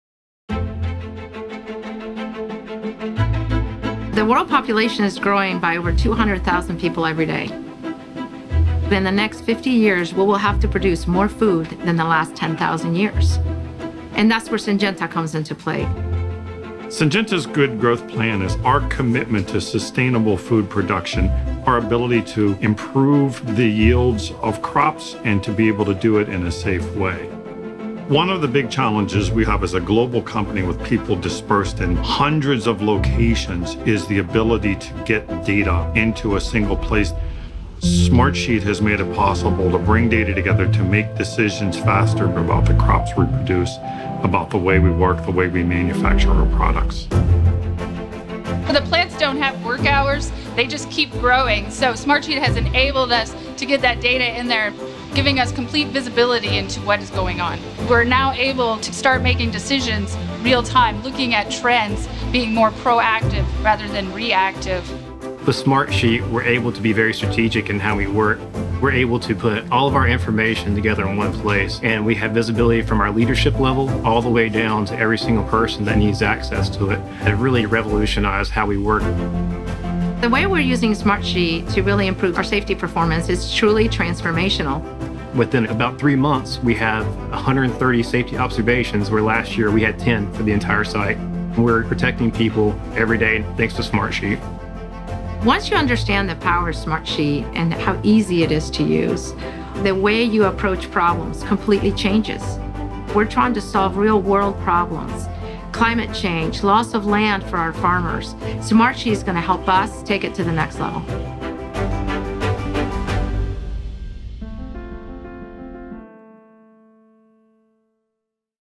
How Syngenta makes data-driven decisions with Smartsheet
Smartsheet helps Syngenta manage agricultural research and product development in more than 90 countries.